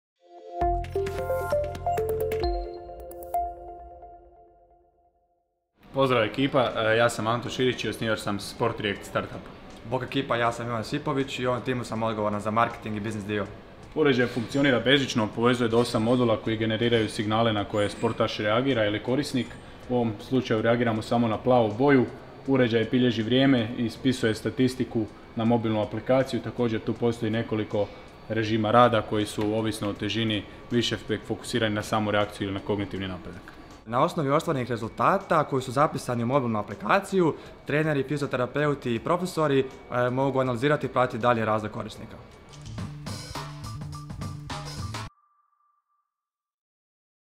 Predstavljamo timove pete generacije Startup Factory predakceleracijskog programa: Sport React
Upoznajte naš sportski startupfactory tim Sportreact čiji će vam IoT uređaj pomoći u razvoju reakcija i testiranju sposobnosti. Predstavljaju ga Anto Širić i Ivan Josipović.